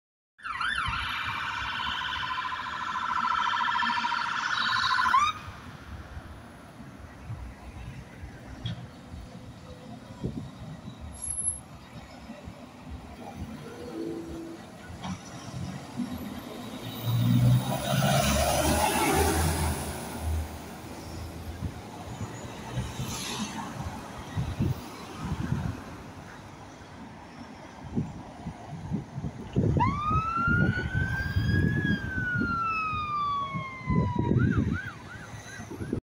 Derbyshire Fire & Rescue - Unimog Animal/Bariatric Rescue Unit
Alfreton's Animal/Bariatric rescue unit seen responding through Chesterfield.